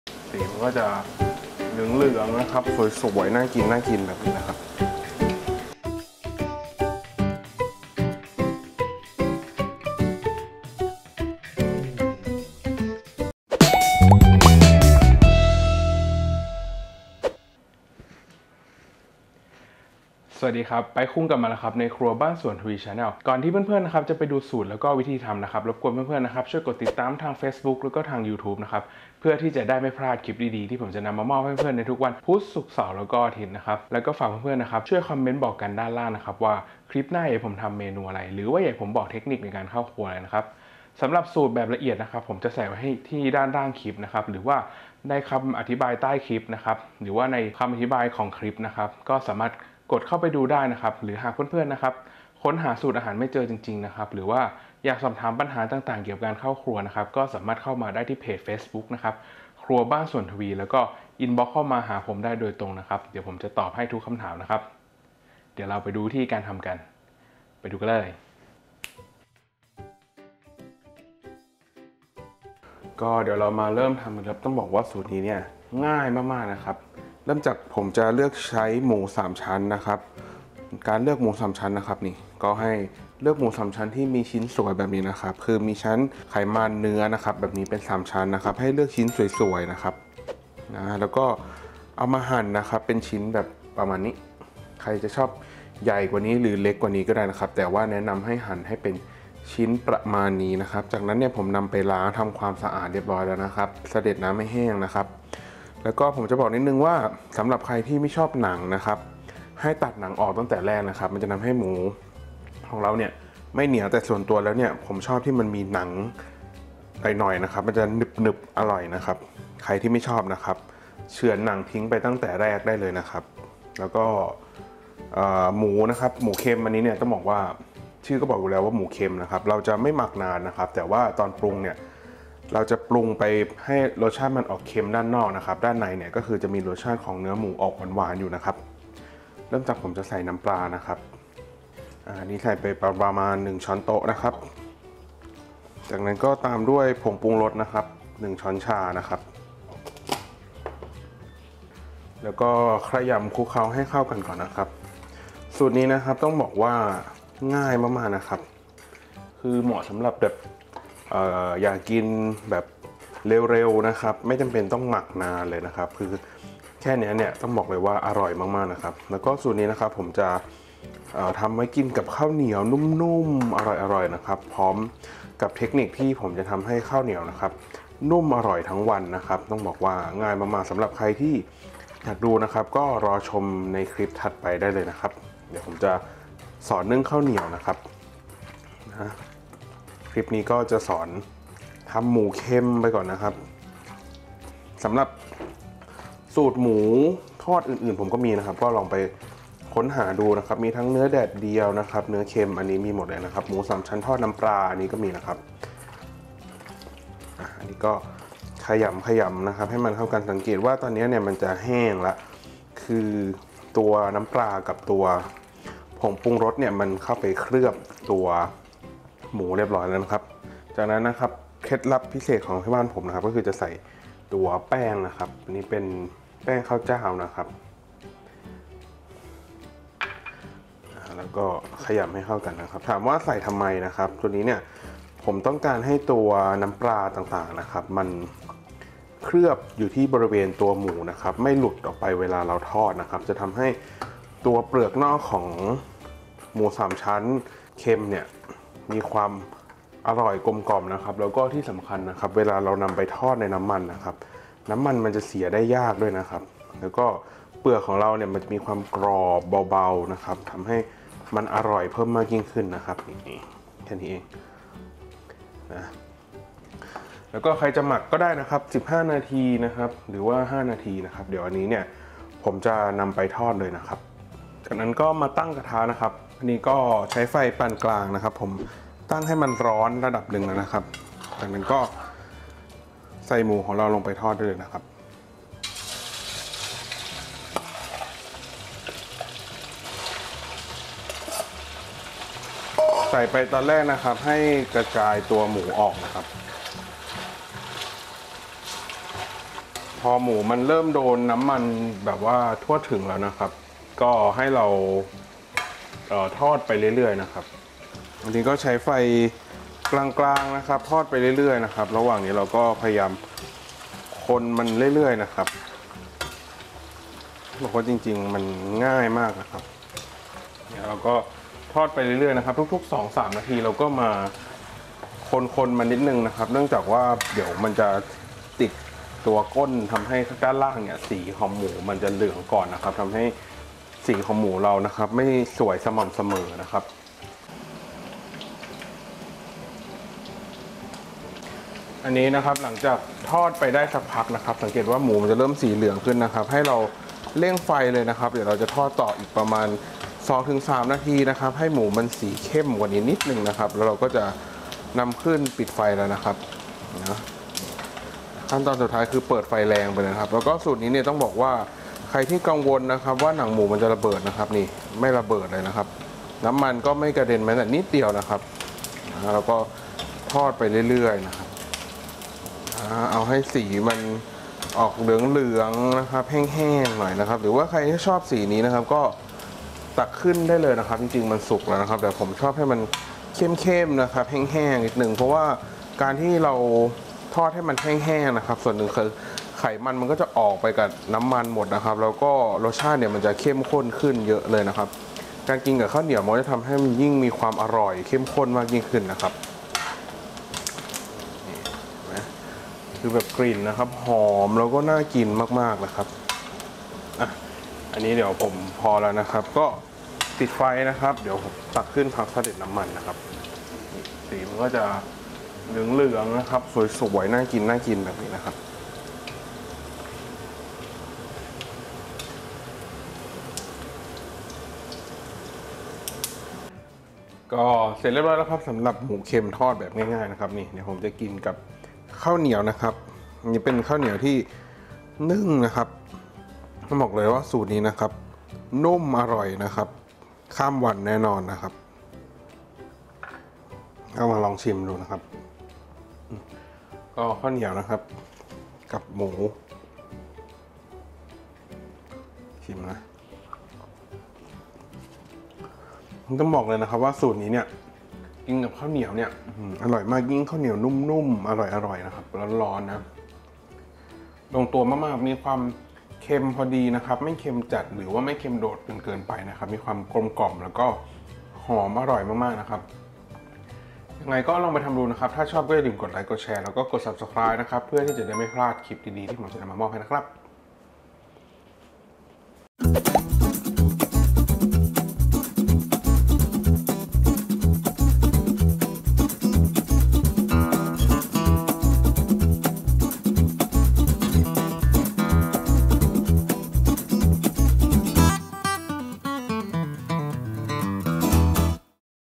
หมูสามชั้นเค็ม หมูสามชั้นทอด หอมนุ่ม อร่อย สูตรครัวบ้านสวนทวี ทำง่ายมาก ใช้เวลาเพียง 15 นาทีได้กิน
ถ้าชอบอย่าลืมกด Subscribe และกดกระดิ่ง เพื่อไม่พลาดคลิปออกใหม่ด้วยนะครับ ผมจะได้มีกำลังใจทำคลิปต่อไปครับ

หมูสามชั้น 300 กรัม
แป้งข้าวเจ้า 1 ช้อนโต๊ะ